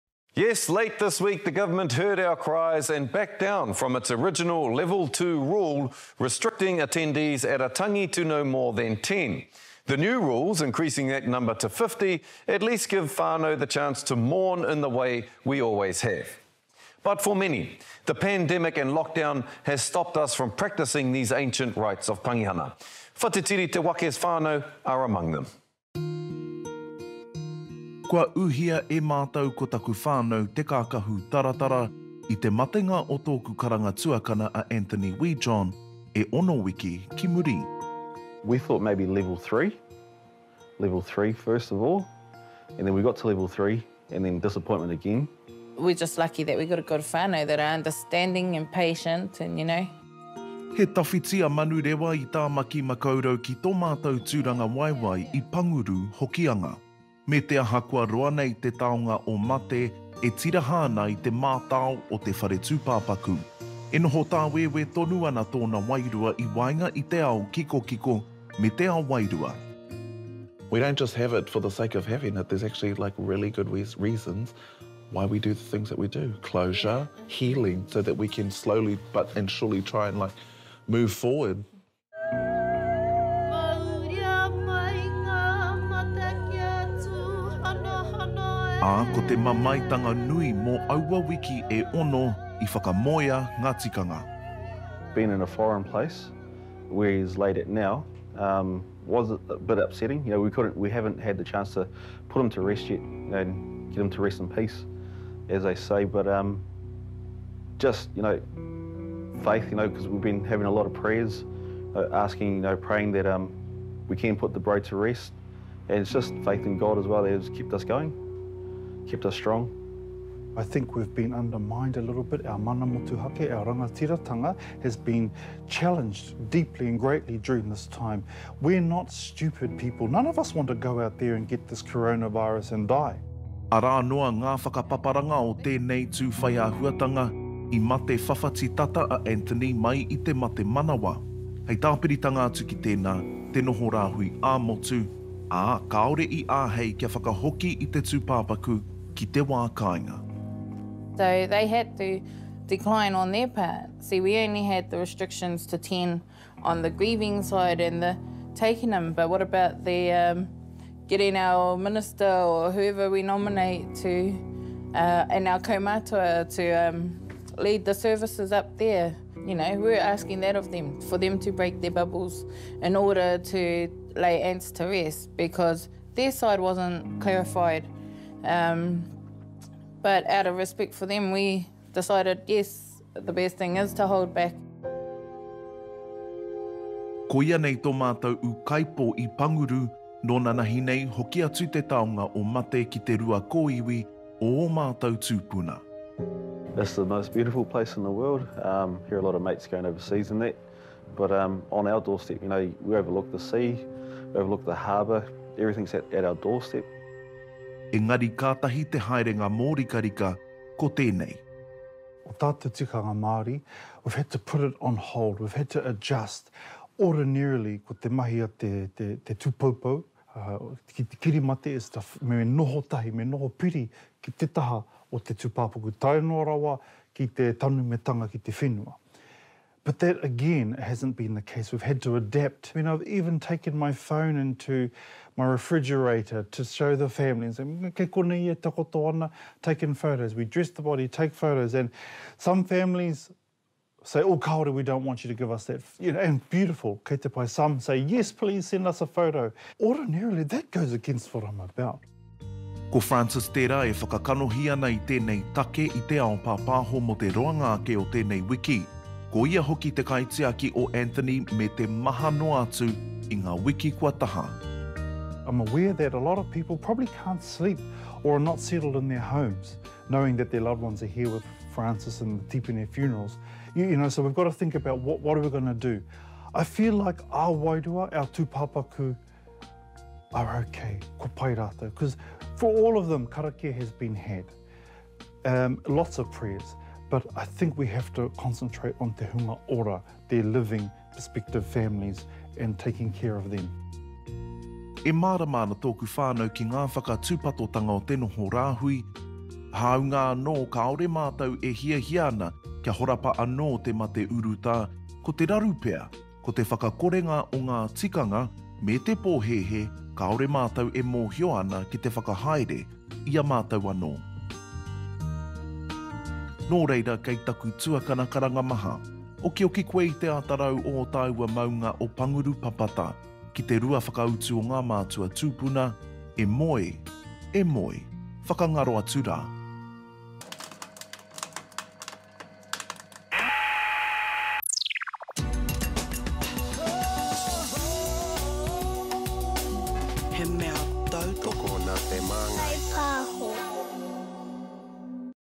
Tangi (funeral) during Covid-19 lockdown
Late this week the government heard our cries and backed down from it’s original level 2 rule restricting attendees at a tangi (funeral) from no more than 10 to 50. For many, the pandemic and lockdown has stopped Māori from practicing these ancient rites. Whatiriti te wake’s whānau are among them.

Location: New Zealand
Reporter: Whatitiri Te Wake